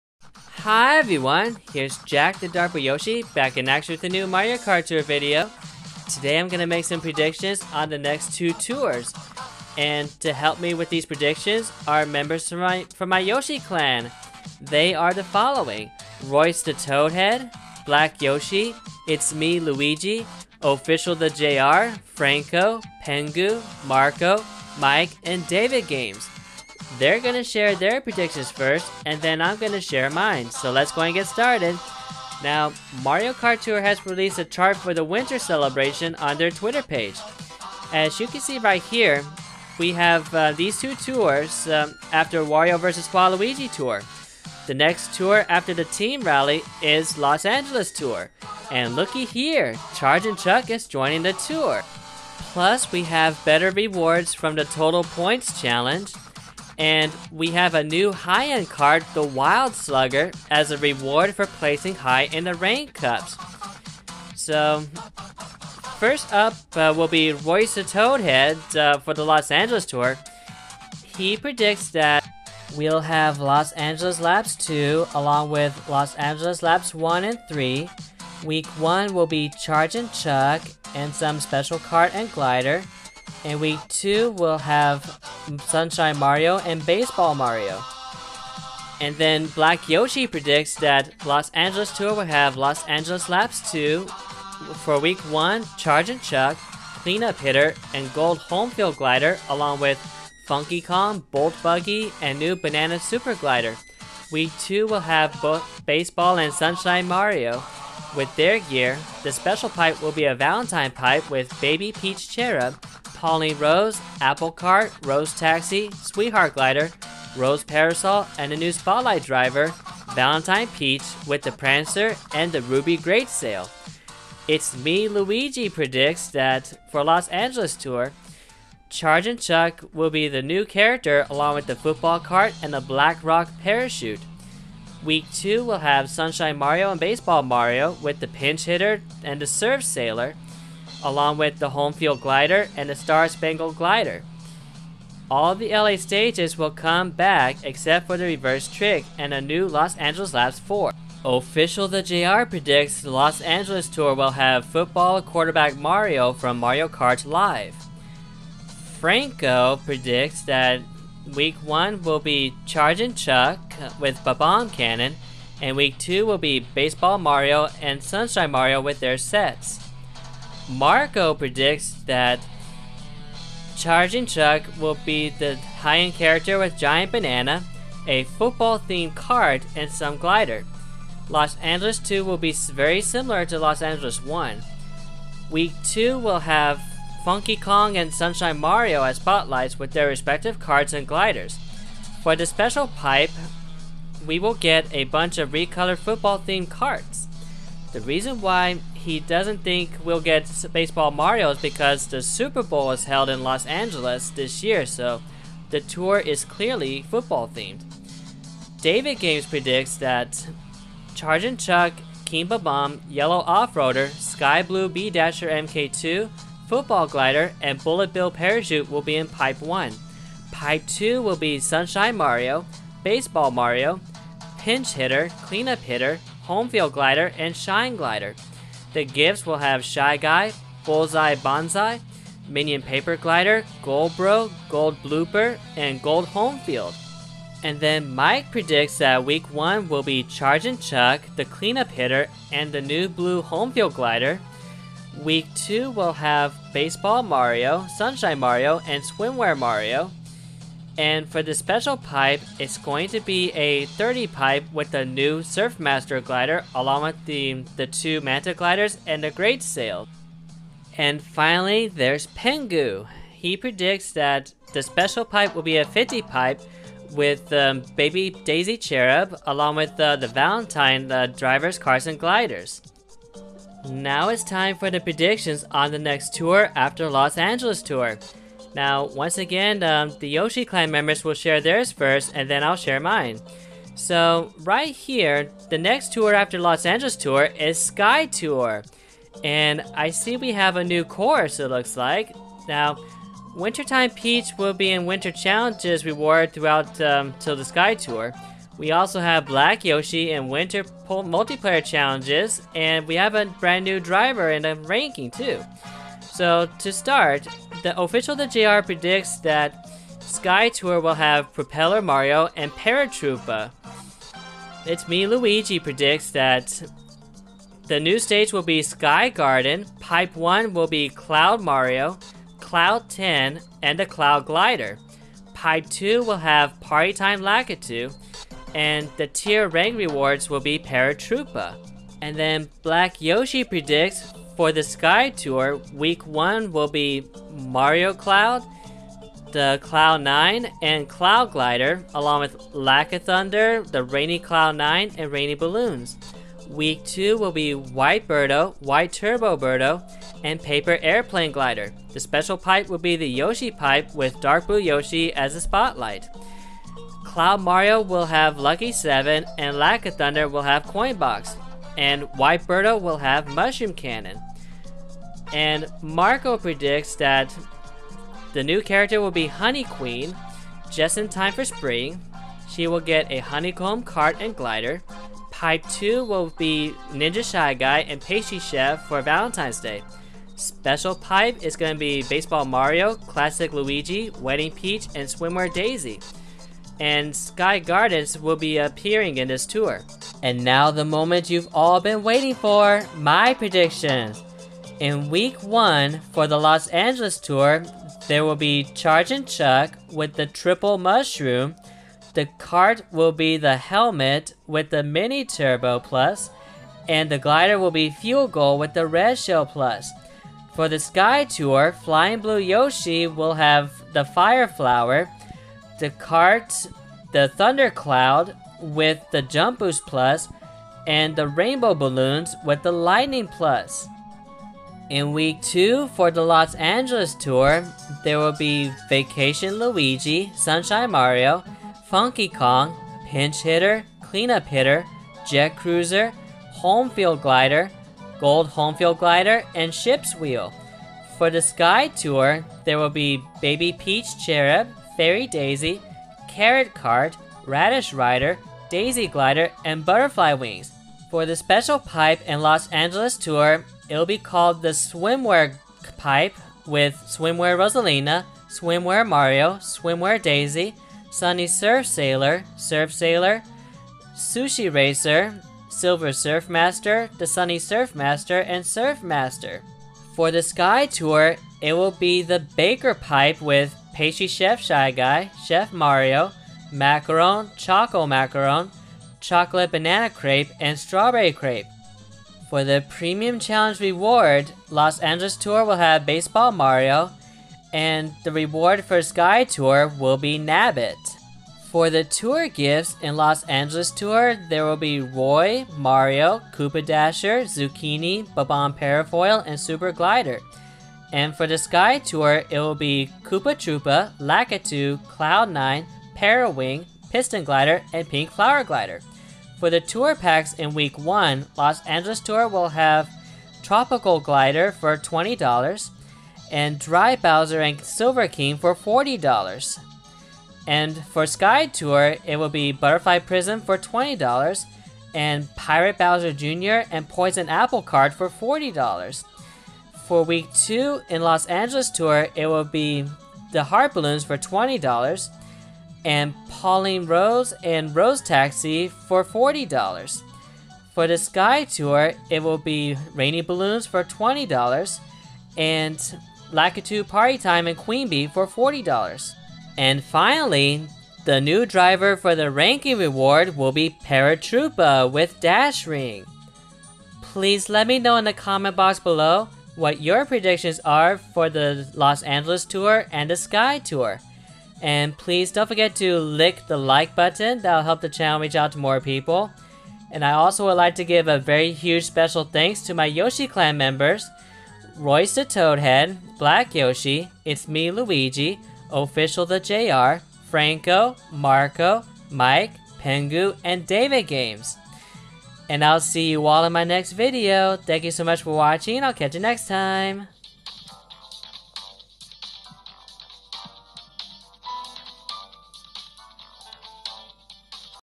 Los Angeles Tour and Sky Tour EARLY PREDICTIONS | Mario Kart Tour (ft. Yoshi Clan)
In this video, I share my early predictions along with a few of my Yoshi Clan discord members for the Los Angeles Tour and Sky Tour. Hope you'll enjoy, and please don't forget to "lick" that Like button.